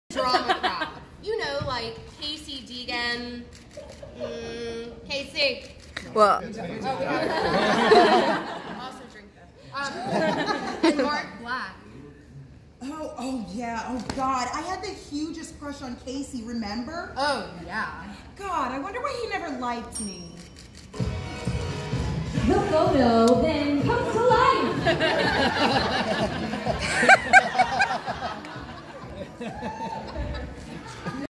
Casey Deegan and Mark Black
Scene from A Drinking Game NYC presents Romy and Michele's High School Reunion